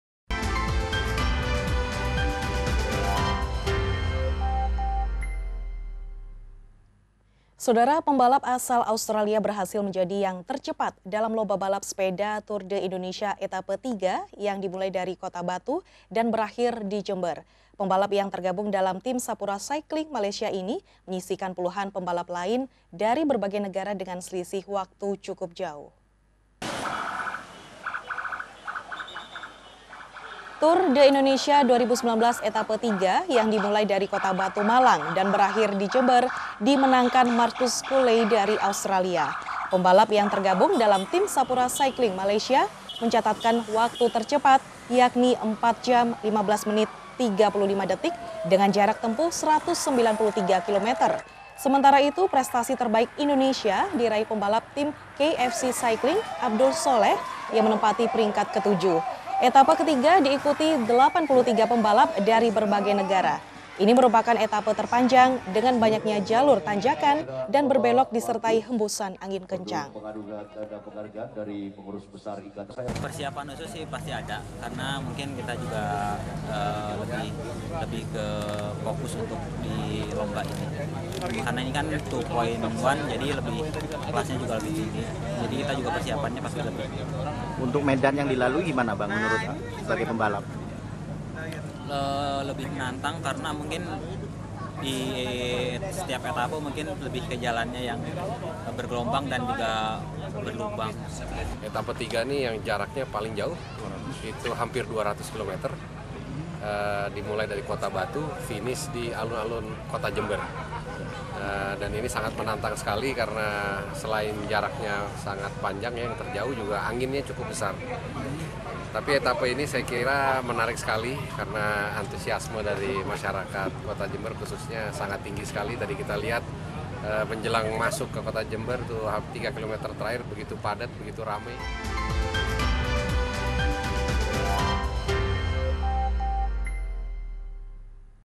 PEMBALAP AUSTRALIA JUARAI TOUR DE INDONESIA ETAPE KETIGA_KOMPASJATIM
PEMBALAP ASAL AUSTRALIA BERHASIL MENJADI YANG TERCEPAT DALAM LOMBA BALAP SEPEDA TOUR DE INDONESIA ETAPE TIGA, YANG DIMULAI DARI KOTA BATU MALANG DAN BERAKHIR DI KOTA JEMBER. PEMBALAP YANG TERGABUNG DALAM TIM SAPURA CYCLING MALAYSIA INI, MENYISIHKAN PULUHAN PEMBALAP LAIN DARI BERBAGAI NEGARA DENGAN SELISIH WAKTU CUKUP JAUH.

Jangan lupa untuk menyaksikan live streaming Kompas Jatim dan rangkaian news buletin lainnya dibawah ini :)